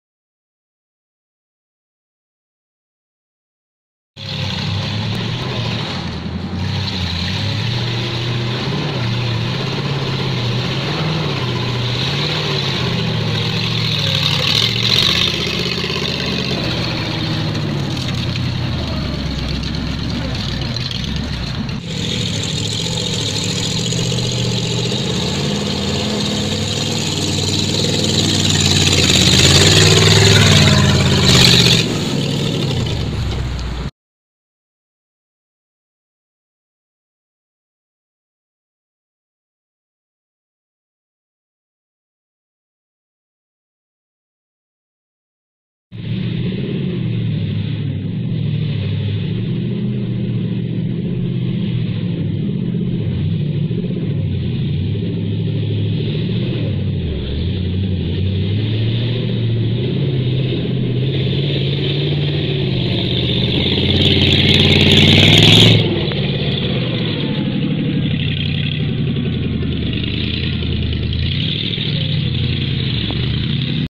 Bren Gun Carrier in the Pines - NZMVC
Lew Skelton checks out the pine plantation.